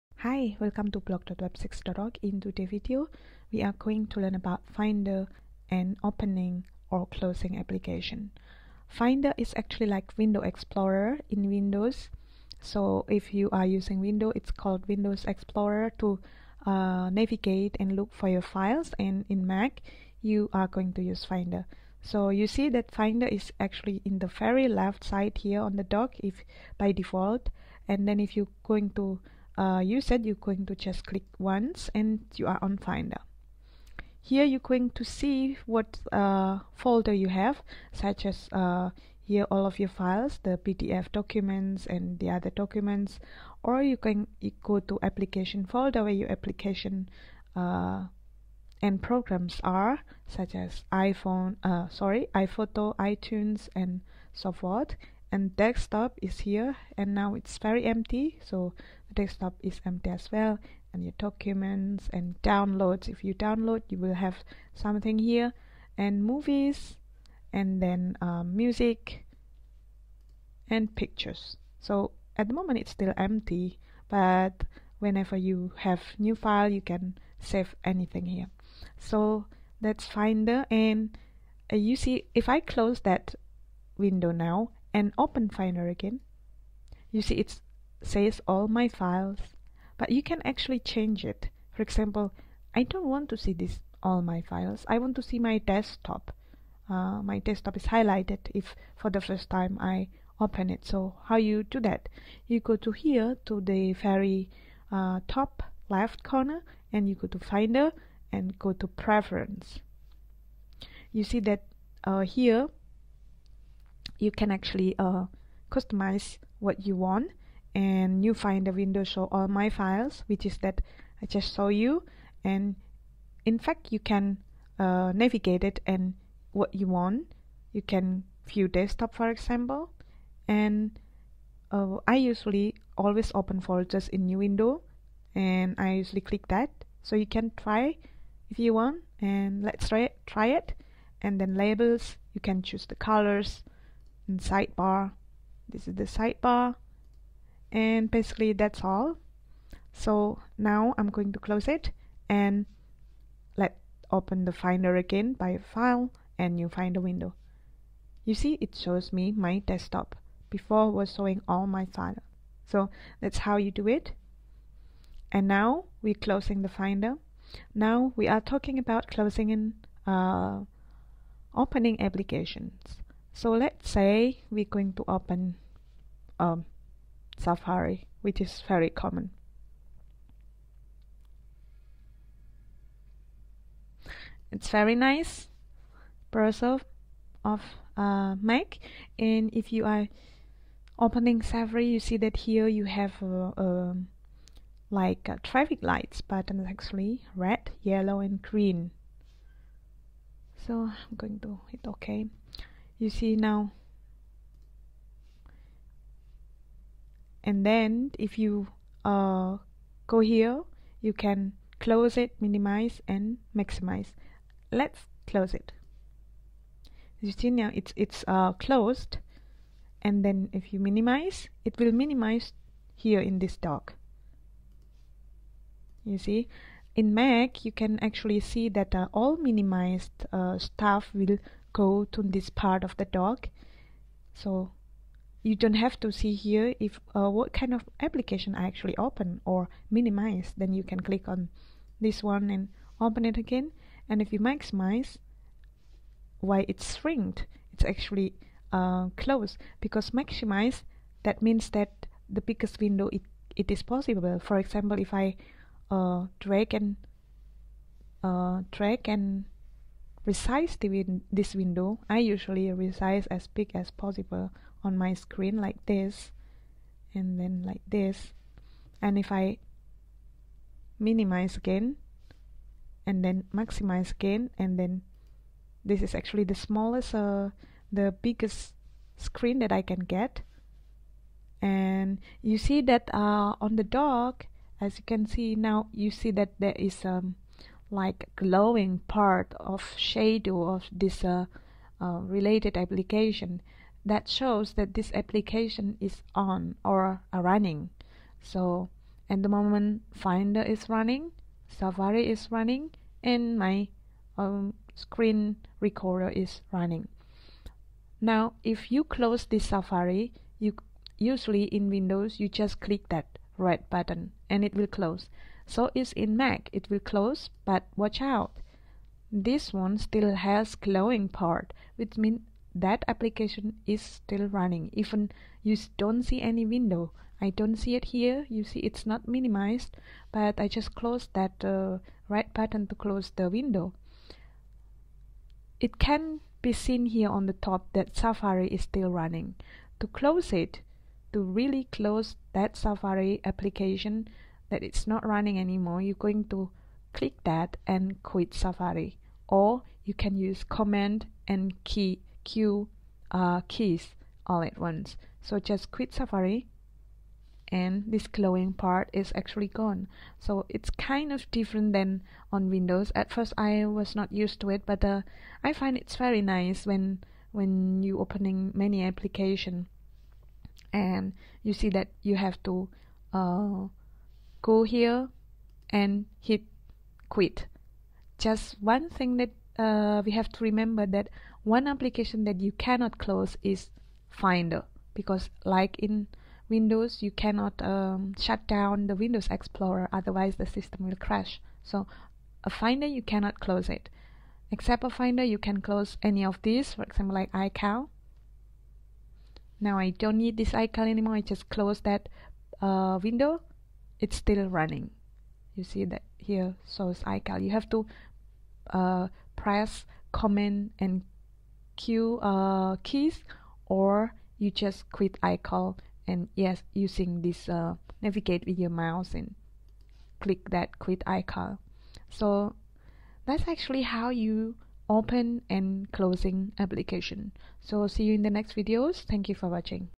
Finder, Open, Close, and Quit Application on a Mac
How to use Finder, open, close and quitting an application in a Mac.